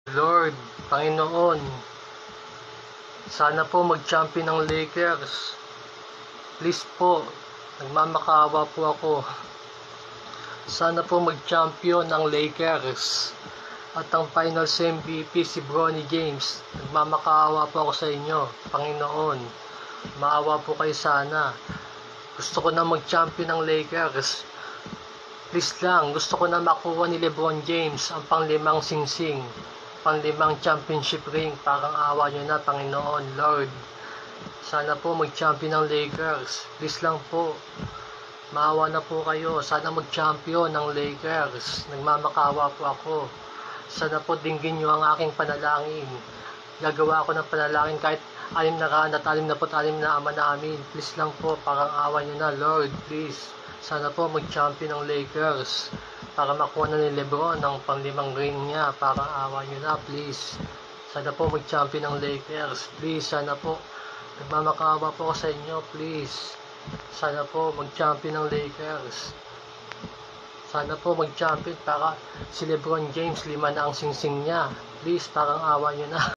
Lord Sana po mag champion Ang Lakers para makuha na ni LeBron James Ang kanyang 5th ring please
Panginoon Sana po mag champion Ang Los Angeles Lakers para po makamit na ni LeBron James Ang kanyang 5th Ring nagmamakaawa PO ako sa inyo Lord araw araw PO ako magdadasal para Lang po mag champion Ang Lakers Parang awa nyo Napo please 🙏